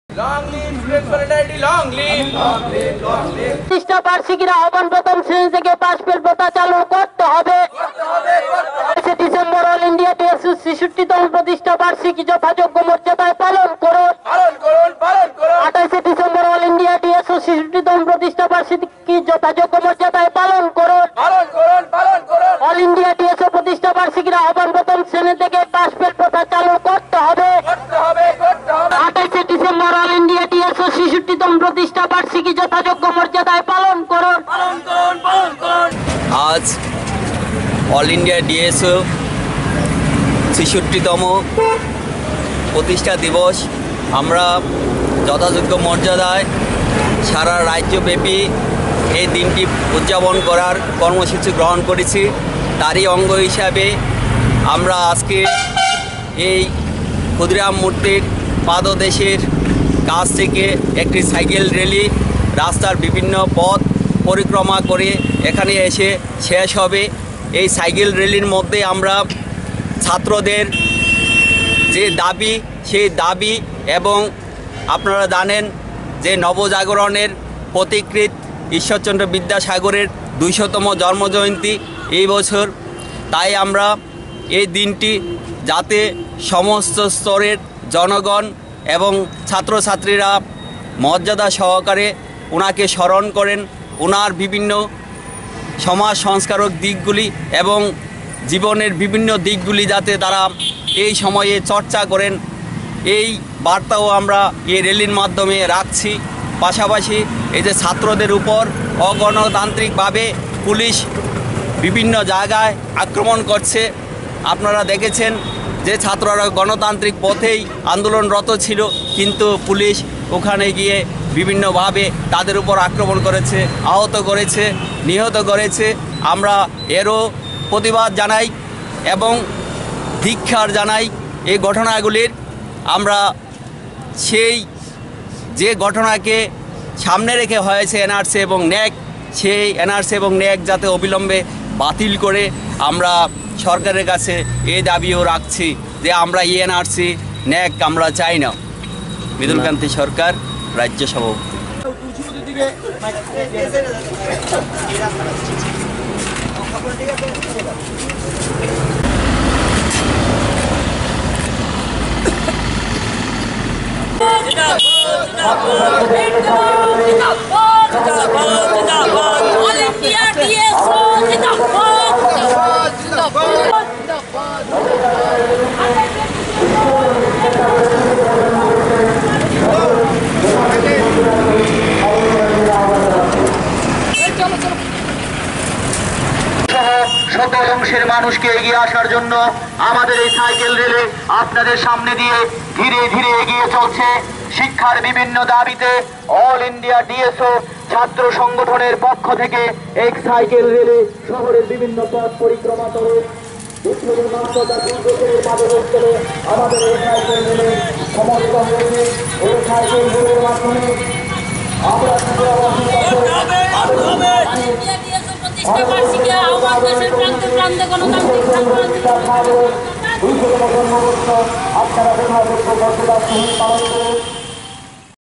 DSO holds mass cycle rally seeking re-introduction of the pass-fail system from class 1
DSO holds mass cycle rally seeking re-introduction of the pass-fail system from class 1 at Agartala on December 28